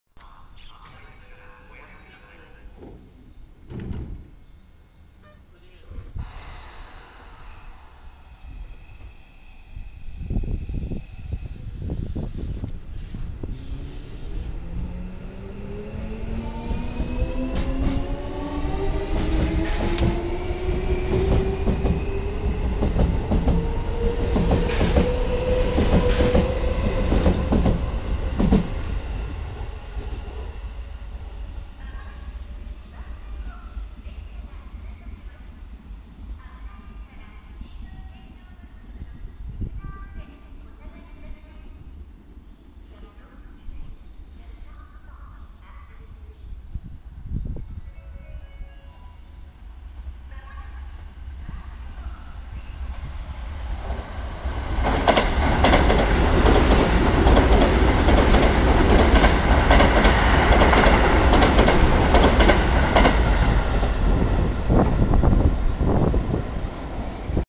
京王線8000系各駅停車京王八王子行と、8000系を撮った。長沼駅
長沼駅にて
I took this video of the Keio Line 8000 series, which stops at all stations, bound for Keio Hachioji, and the 8000 series. Naganuma Station